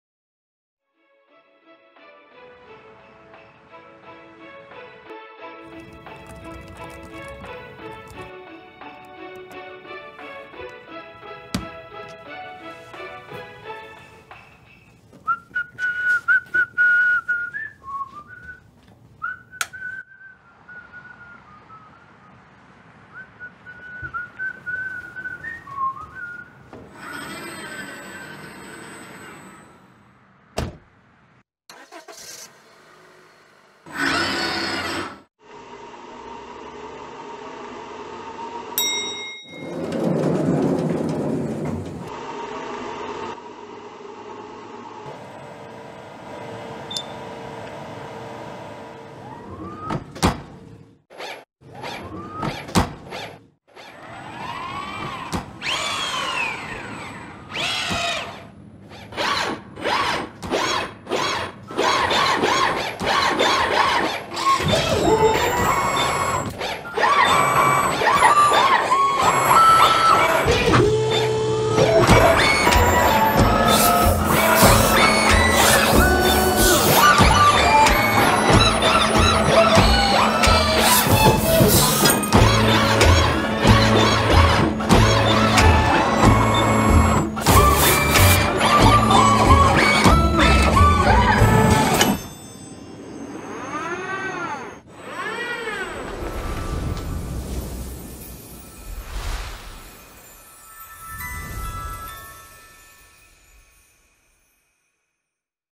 Fanuc karácsony 2021
A Fanuc robotjai így hangolódnak a karácsonyra.